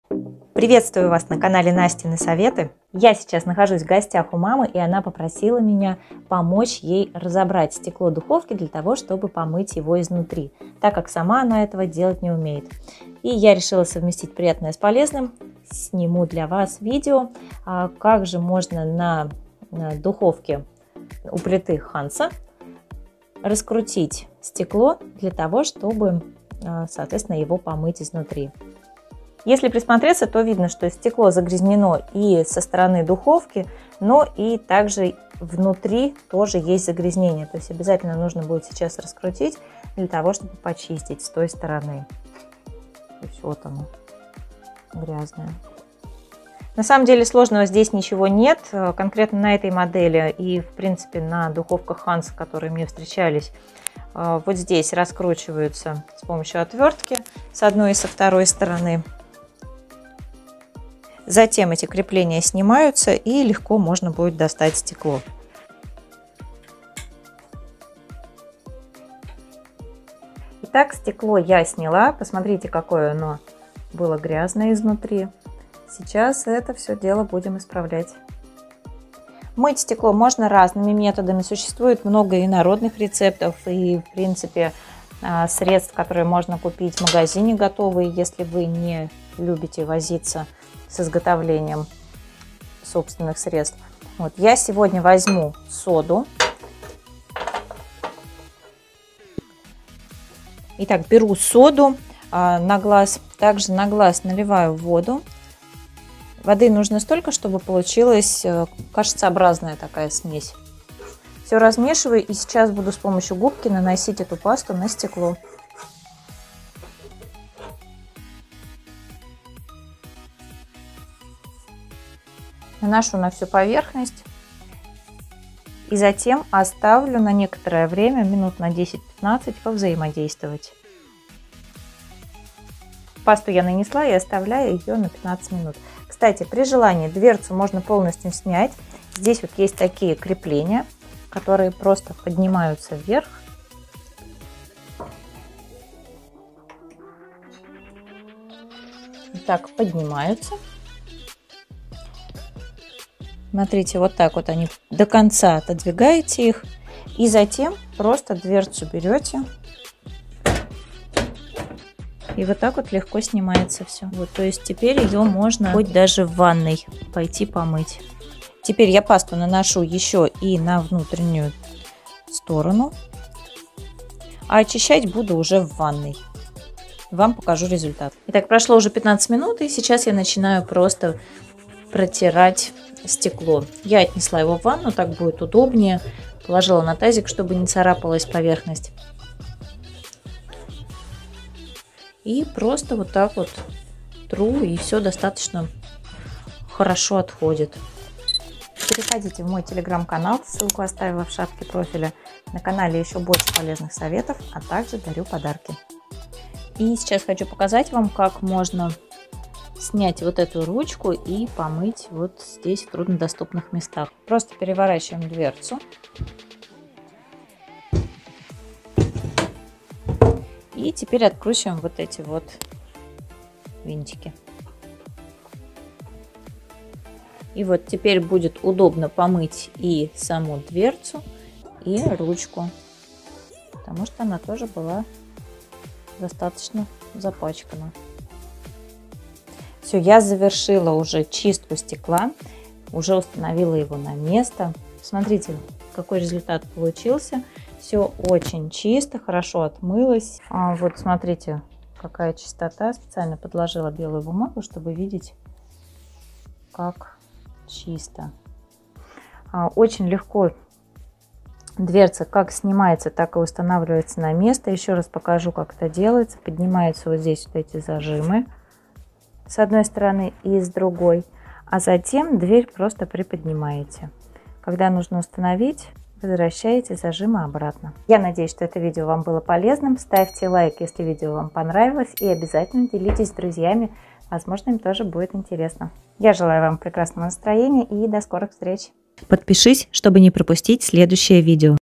Как снять и очистить стекло духовки от жира и нагара: самый простой способ
Мало кто знает как снять стекло духовки. Практически у всех современных духовых шкафах можно снять и дверцу, и стекло. Это облегчает чистку.

На примере духовки Hansa показала в видео как легко можно разобрать и обратно собрать дверцу и почистить стекло со всех сторон.

Если у вашей духовки другие крепления, то просто посмотрите в инструкции как снять стекло.

Благодарю за внимание. Буду рада, если кому-то помог этот ролик.